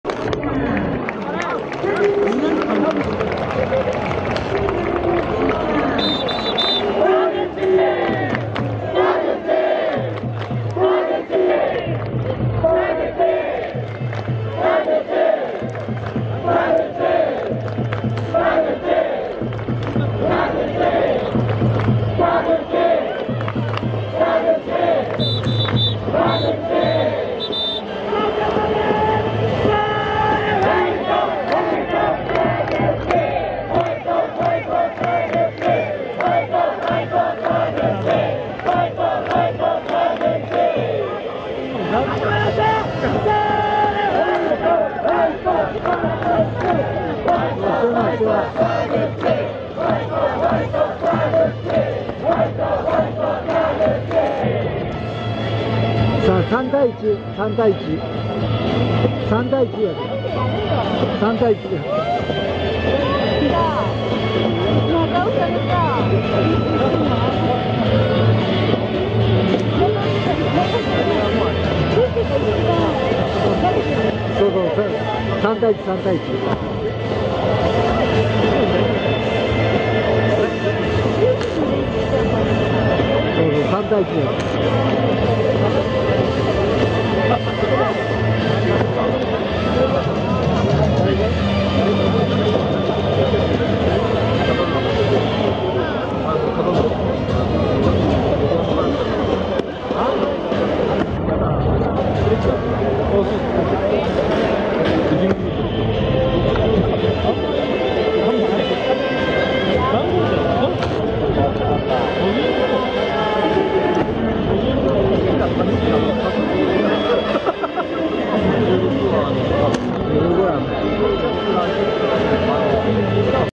《最終戦の意地9回2アウト 34田口麗斗 ダメ押し阻止に行く》 楽しそうな外野手
2023/10/4 神宮球場
東京ヤクルトスワローズ
阪神タイガース《25回戦》

《ホーム最終戦の意地》
1点ビハインド9回表
たくさん雨が降る中で
2アウトから
マウンドに向かってゆく
スワローズ意地の場面

マウンドもブルペンも
スパイクの裏は泥だらけ
この緊張の場面に外野手は
なんだか楽しそうな雰囲気

レフトスタンド全開放
阪神ファンが埋め尽くす